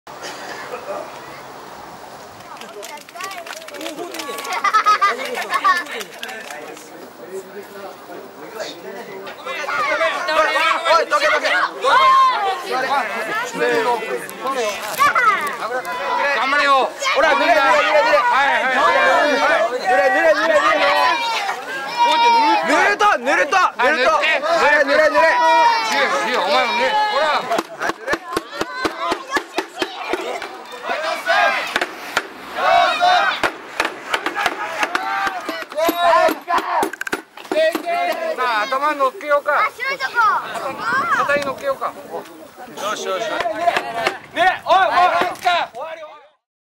泥だらけになるほど豊作に 奇祭「泥打ち祭り」 福岡県朝倉市の阿蘇神社
福岡県朝倉市の阿蘇神社で３月２５日、子どもたちが氏子から選ばれた代宮司に泥を投げつけて豊作を祈願する「泥打ち祭り」があった。県の無形文化財に指定され、江戸時代から続くとされる奇祭。体に泥が多くつくほど、その年は豊作になるといわれる